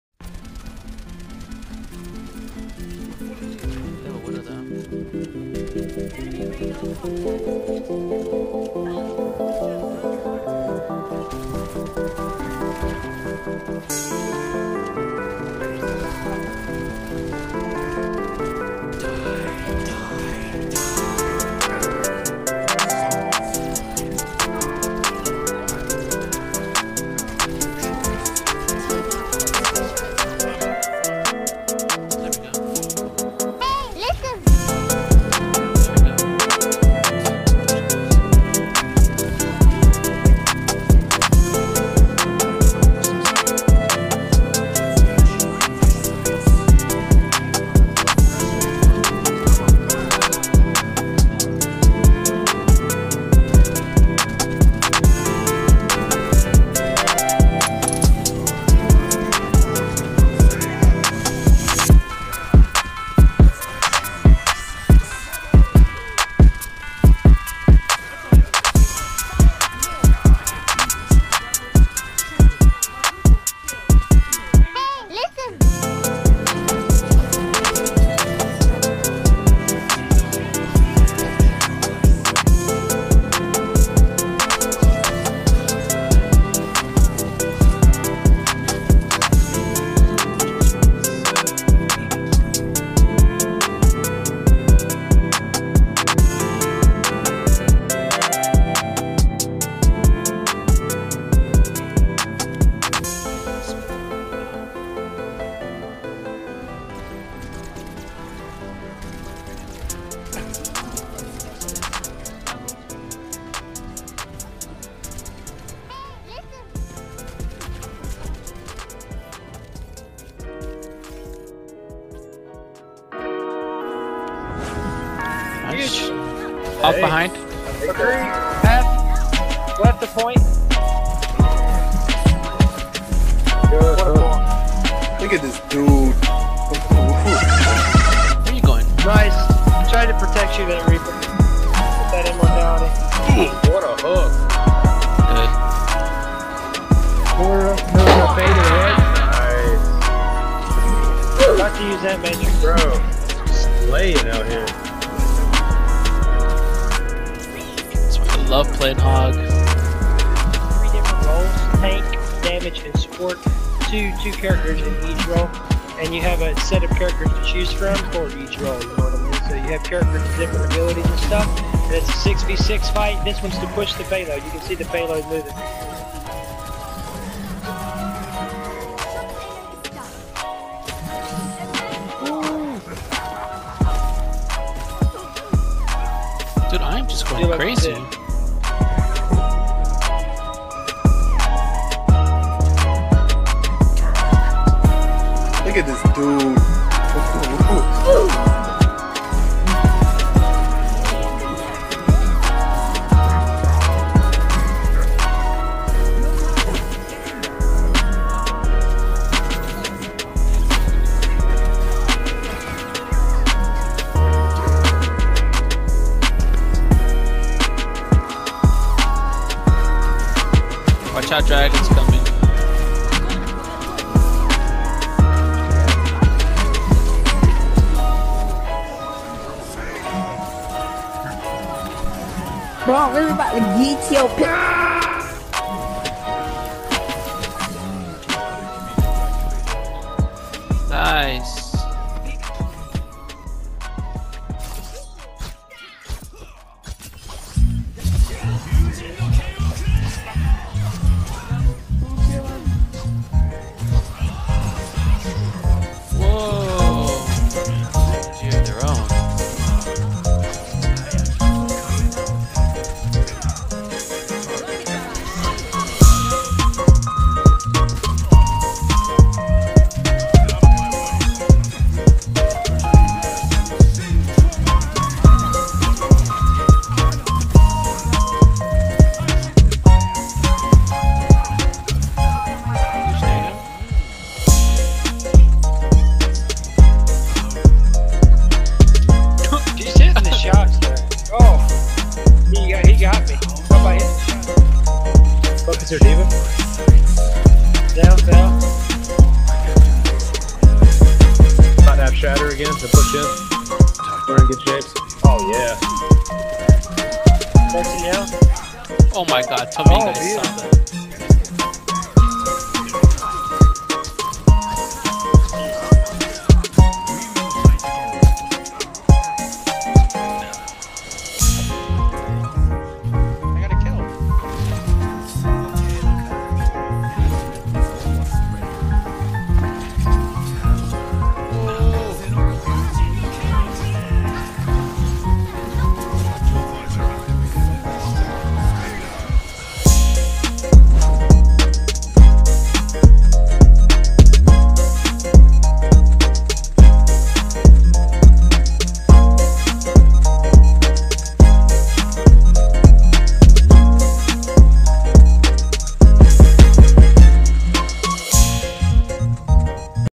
The Best R3APZz Carry
A great time grinding in competitive with 2JReplays and others.